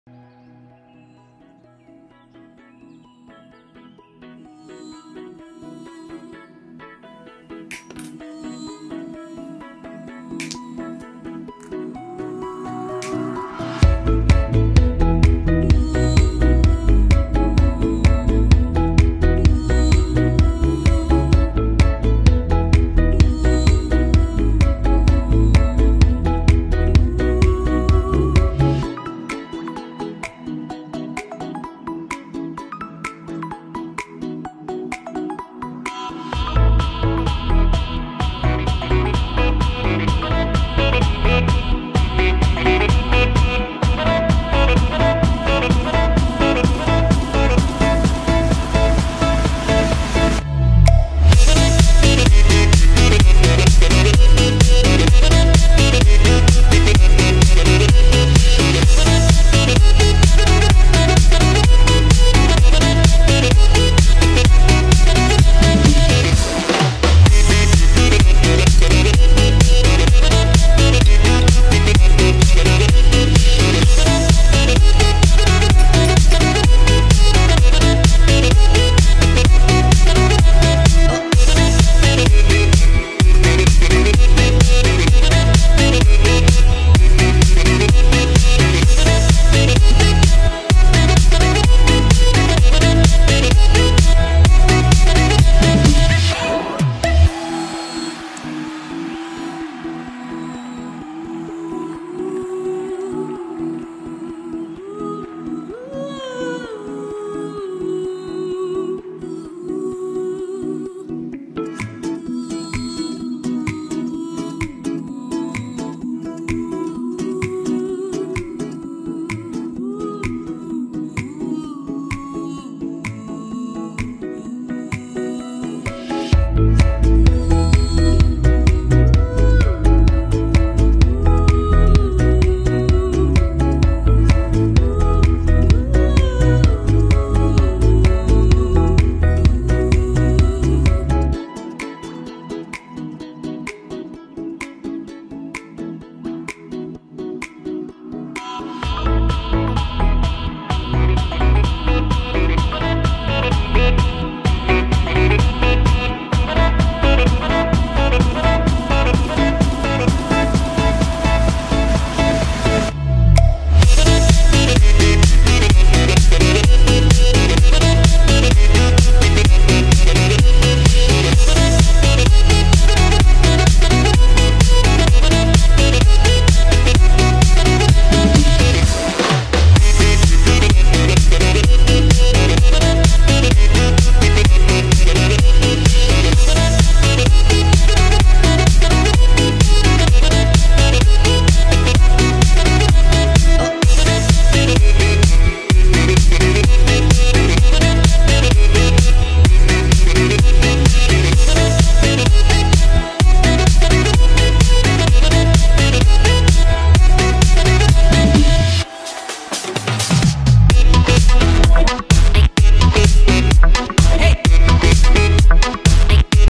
Free energy electricity generator using Lemon. Self running light Bulb 2018
In this video you see How to Make Free Energy Electricity Generator using Lemon and self running light Bulb 2018